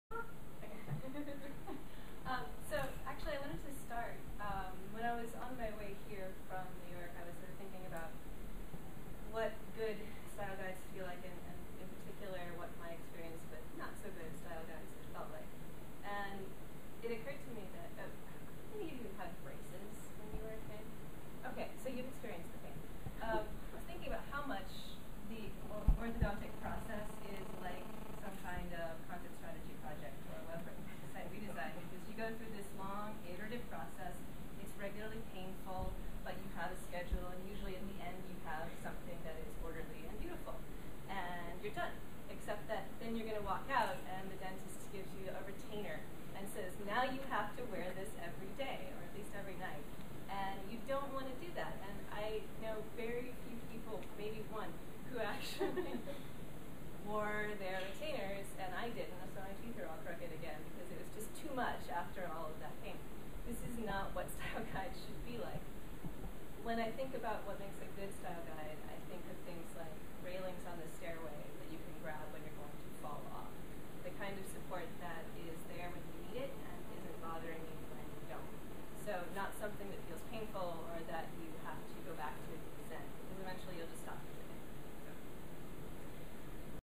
Content Strategy New England: Mandy Brown and Erin Kissane - Style Guide as Retainer
Nov. 15, 2010 - Microsoft NERD
A Pragmatic Guide to Editorial Style